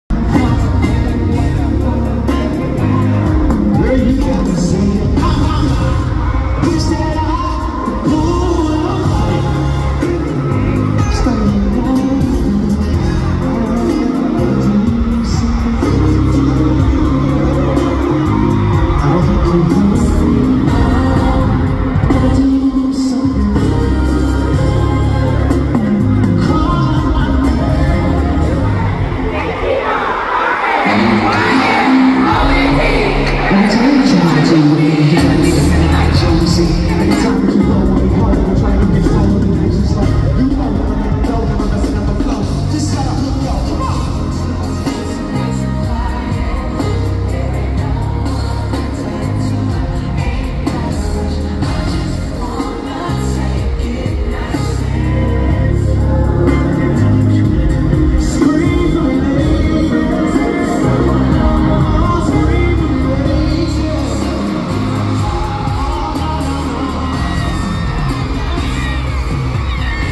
Usher Past Present Future Concert (Detroit)
Usher's performance at Detroit's Little Caesars Arena was exceptional. His sharp and stylish appearance, combined with his quick footwork and incredible vocals, captivated the audience and had them cheering throughout the entire concert. It was a nostalgic experience that transported us back to the 90s and 2000s